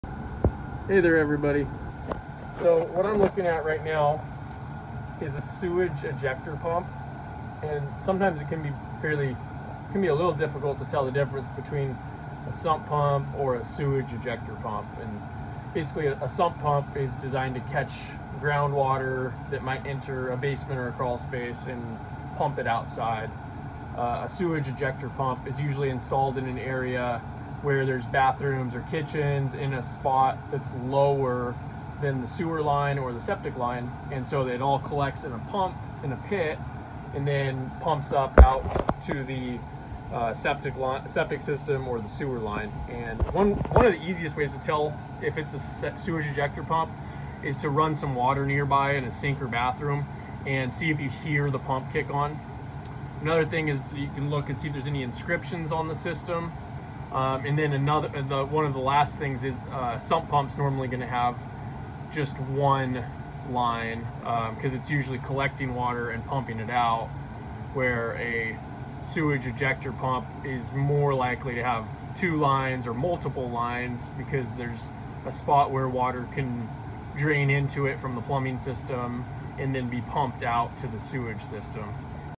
Sewage ejector pump or sump pump?
In this video one of the Allegiance Home Inspectors goes over some of the basic differences between a sewage ejector pump and a sump pump.